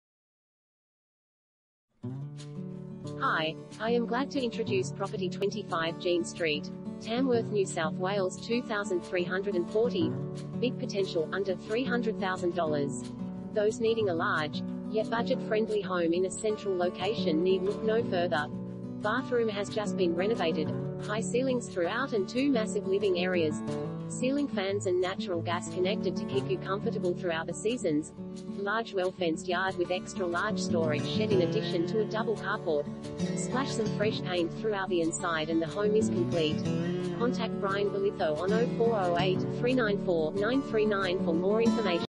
25 Jean St, Tamworth NSW 2340 | AuPropertyReport.Com
25 Jean St, Tamworth NSW 2340
Property Type: house
Bedrooms: 4
Bathrooms: 4
Car Space: 2
Big potential - under $300k